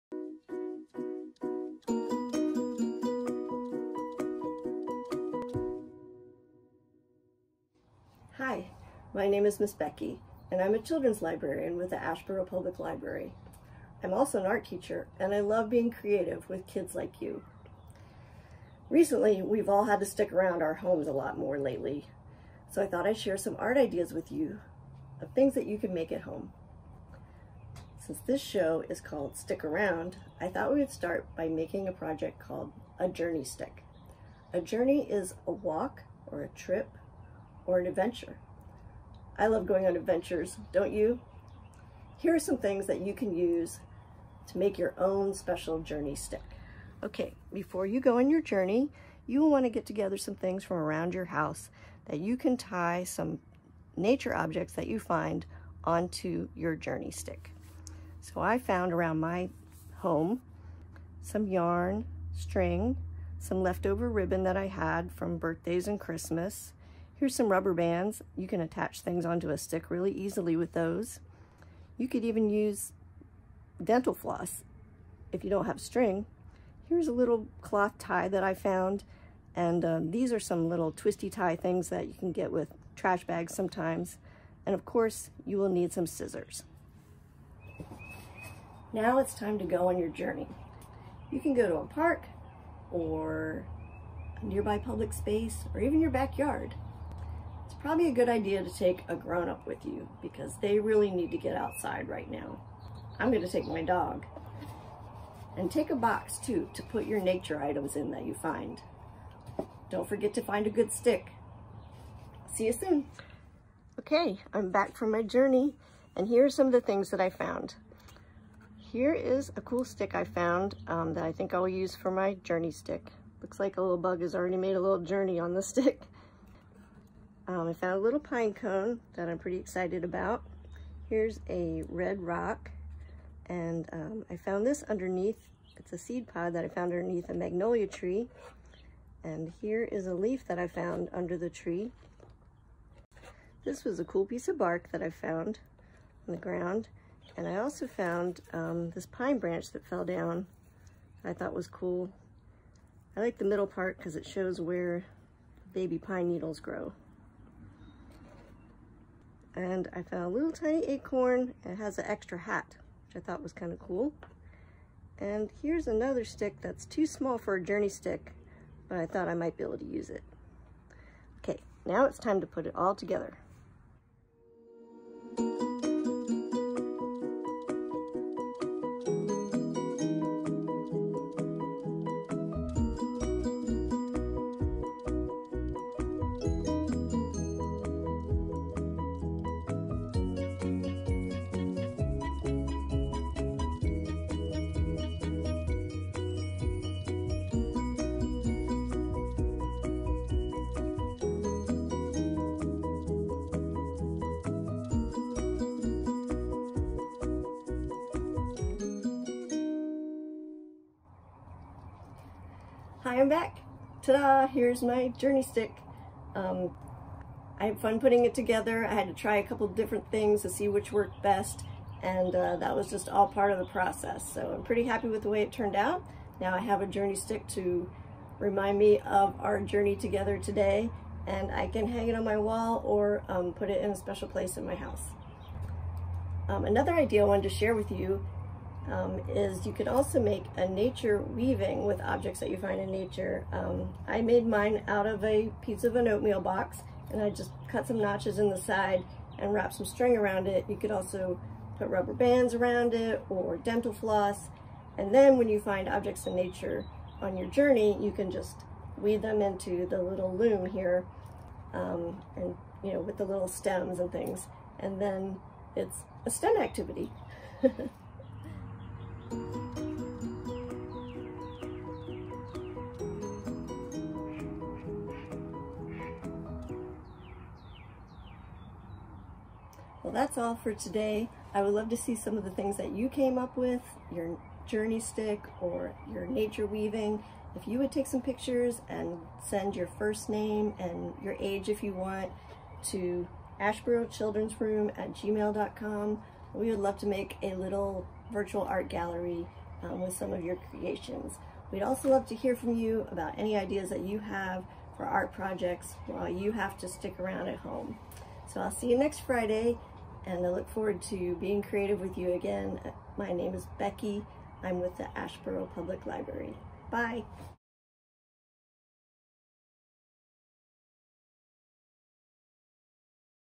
Stick Around with Ms. Becky - Create a "Journey Stick"
Follow along with Miss Becky as she makes a journey stick from items found in nature! You can make your very own journey stick with whatever tools you have at home. All you'll need is a stick, string, and any artifacts you find on your journey.

Stick Around is a video series to facilitate art creation and exploration for children and families at home. New videos posted every Friday!